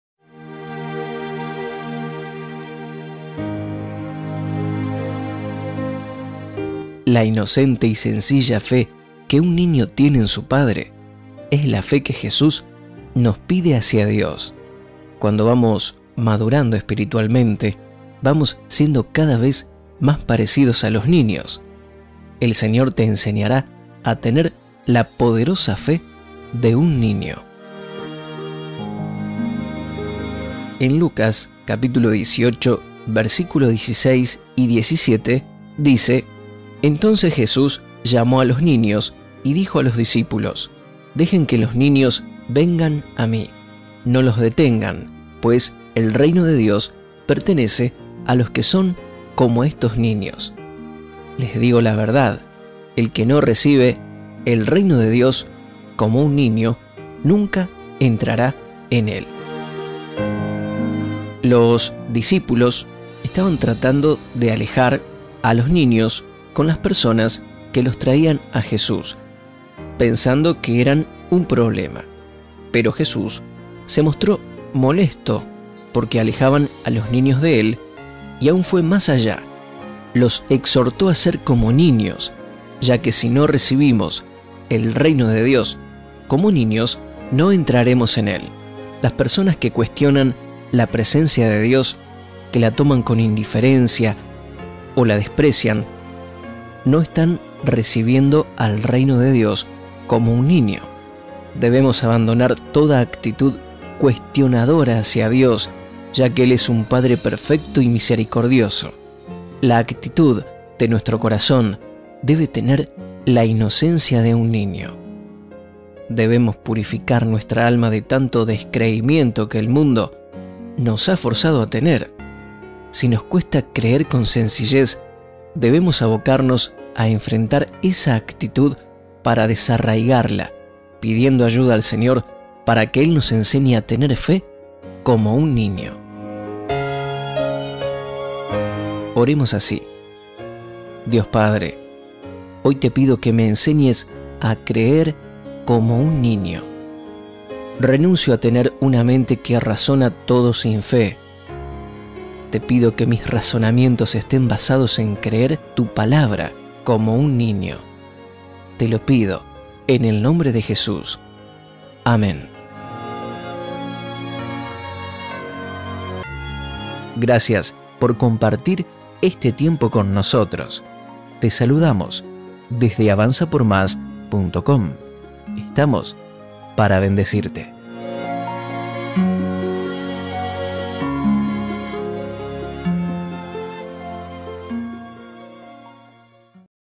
205 - TEN LA PODEROSA Fe de Un Niño Reflexión Cristiana
La inocente y sencilla fe que un niño tiene en su padre, es la fe que Jesús nos pide hacia Dios. Cuando vamos madurando espiritualmente, vamos siendo cada vez más parecidos a los niños. El Señor te enseñará a tener la poderosa fe de un niño.